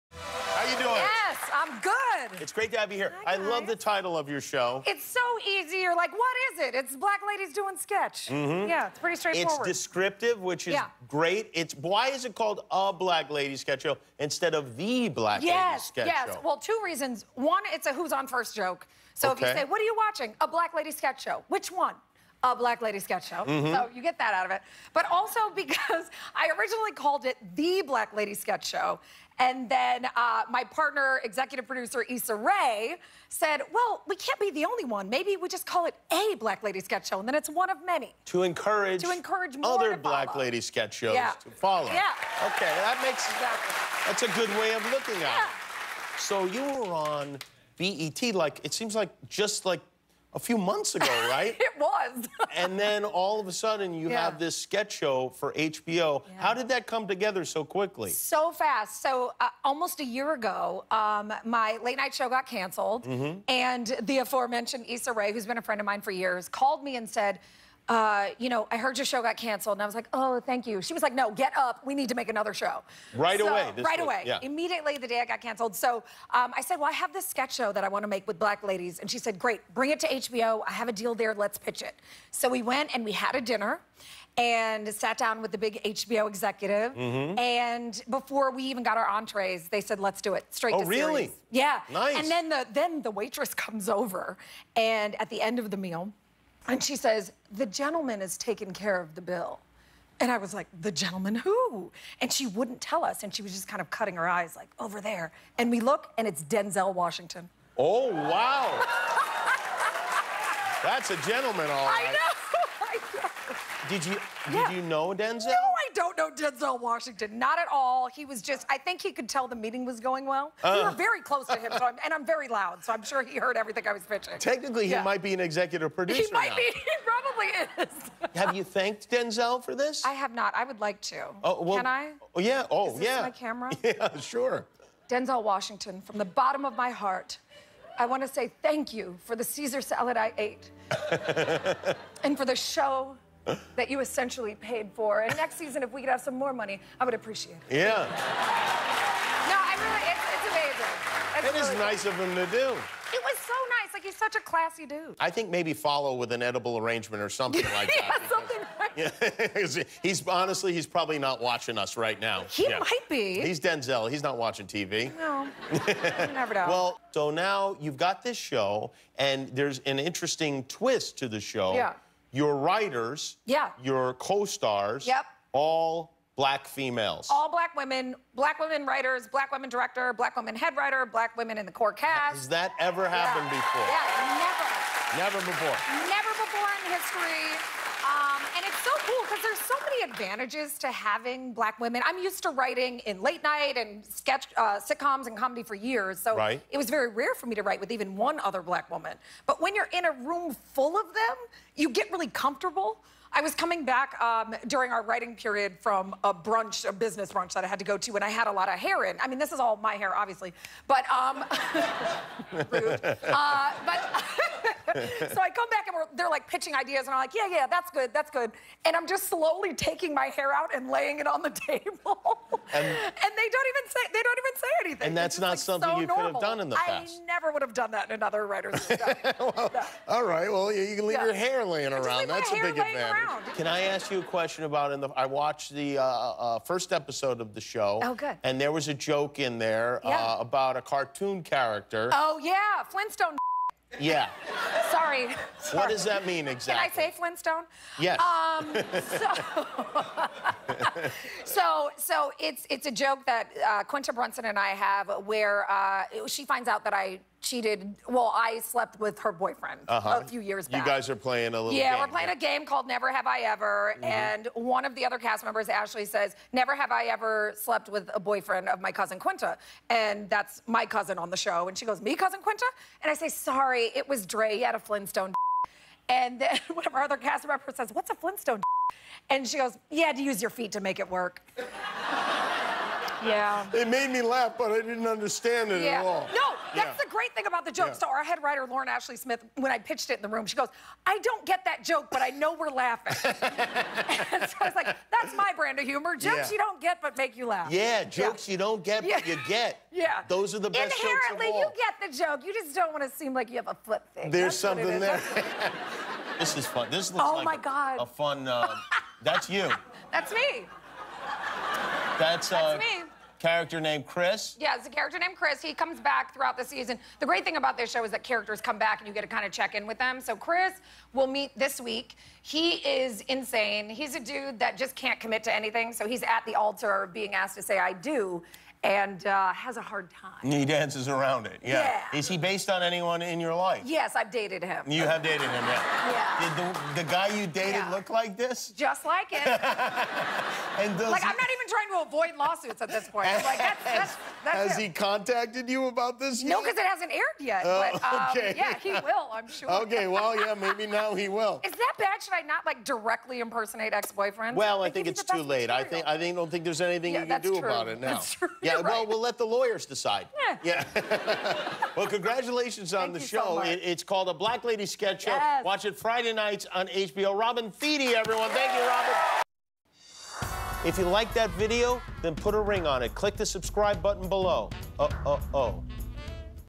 Robin Thede on Denzel Washington & A Black Lady Sketch Show
Robin talks about her new HBO series 'A Black Lady Sketch Show,' and she gives a special thanks to Denzel Washington for paying for her dinner with Issa Rae.